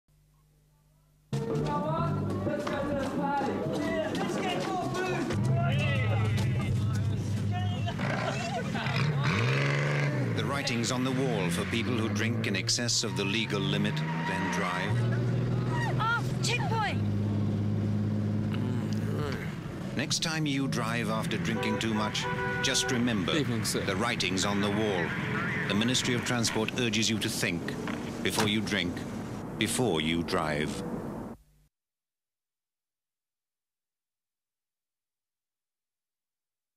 ABPTW4922 173 Writing on the Wall - Random Testing commercial
Ministry of Transport road drink driving commercial from 1984.  Part of a collection of 2 inch video from the Land Transport Safety Authority which Archives New Zealand has preserved.

Source: R21908714 - ABPT/W4922/98398